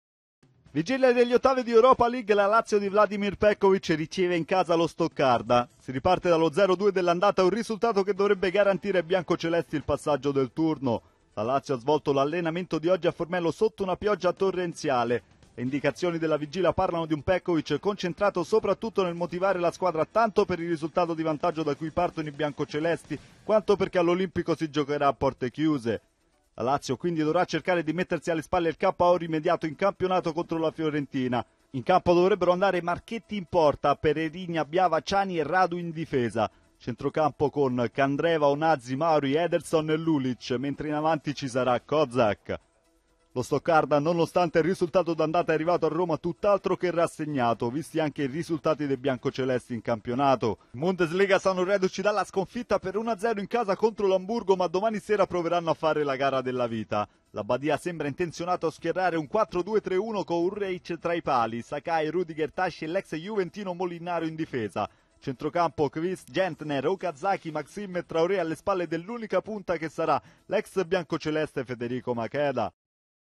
Lazio-Stoccarda: Silenzio, si gioca all'Olimpico
Visitaci su Stasera surreale gara di ritorno della Lazio con i tedeschi: sarà calcio ma senza tifosi.
Silenzio, si gioca all&#39;Olimpico.
Prima delle due giornate di squalifica del campo, ma gli ultras fuori dalla curva Nord faranno sentire il loro "Forza Lazio".